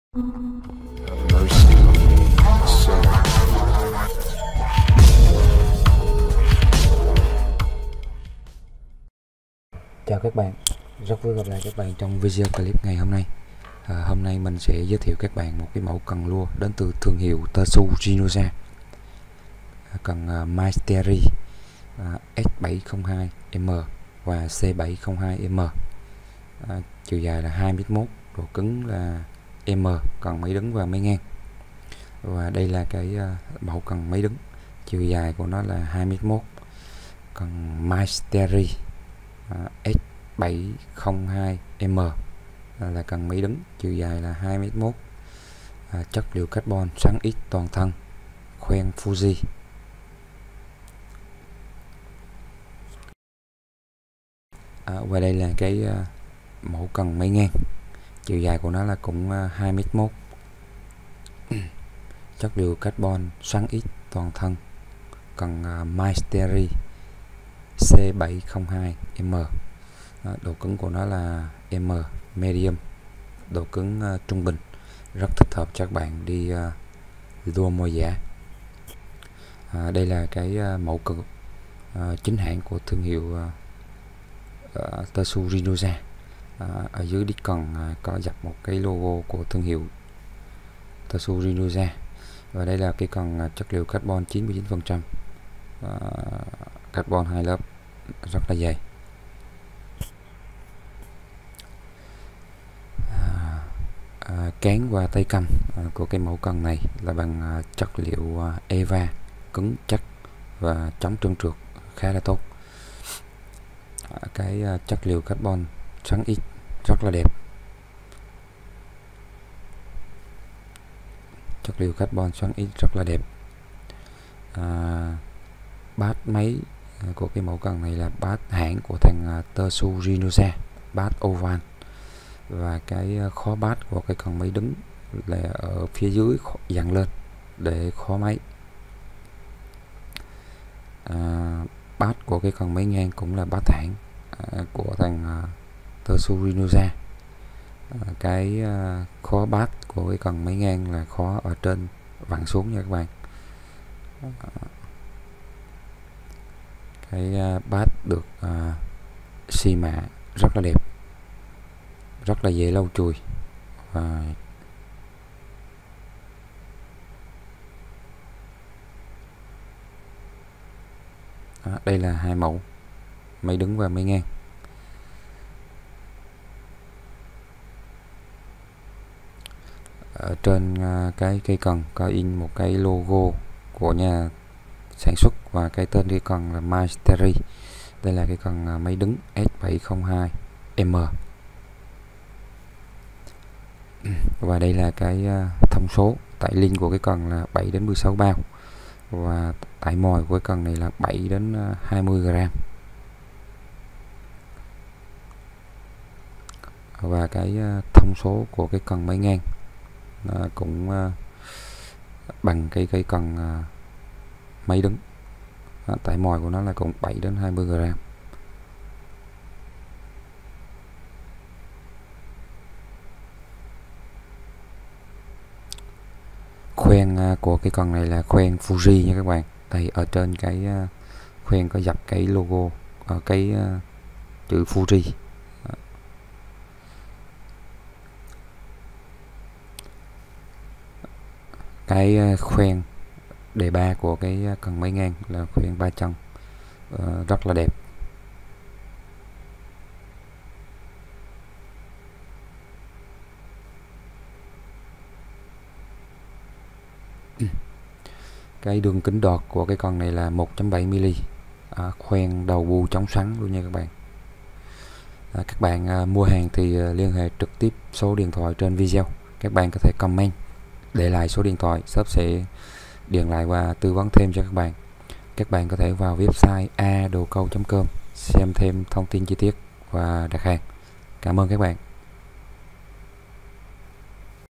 Cần lure Mystery S702M/C702M Carbon 2 lớp
🎣Cần lure Mystery S702M/C702M
💰Giá bán: 1,350,000đ
👉Đường link sản phầm:
Cần lure Mystery C702M / S702M
Mã sản phẩm: Mystery C702M/S702M
Chất liệu: Carbon xoắn X toàn thân
Khoen: Fuji K-Oconcept
Chiều dài: 2M1
Đường kính đọt: 1.7mm
Trọng lượng: 138g
Tải mồi: 7g - 20g
Link: 7-16LB
Thương hiệu: Tsurinoya
🏣Địa chỉ: 1/15 Đường TA05 - Thới An - Quận 12 -  TP.HCM
☎️Điện thoại: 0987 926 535